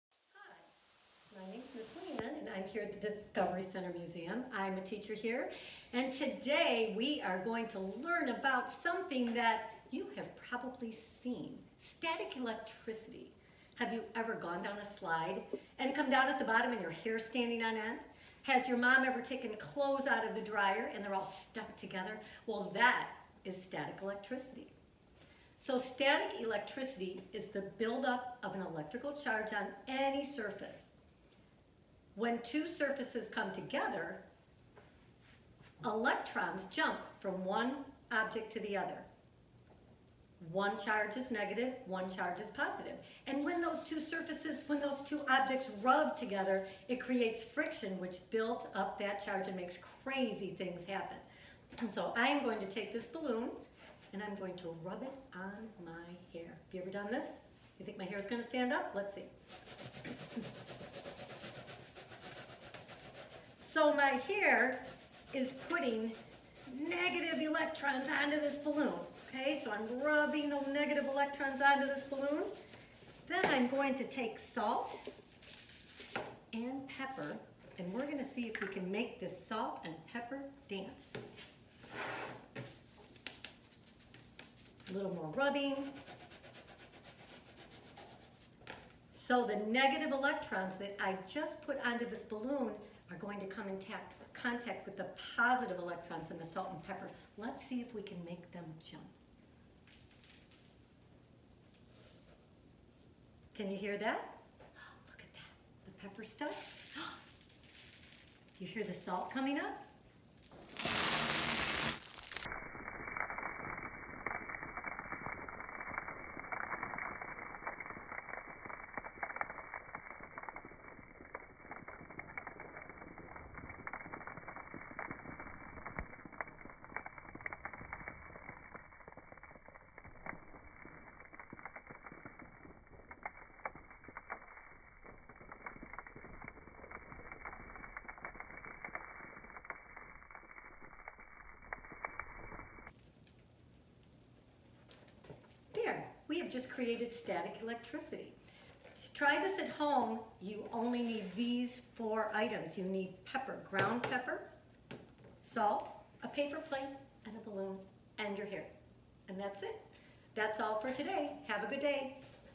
Salt and Pepper Dance - Discovery Center Playful Learning at Home
Observe static electricity first hand with this electrifying experiment!!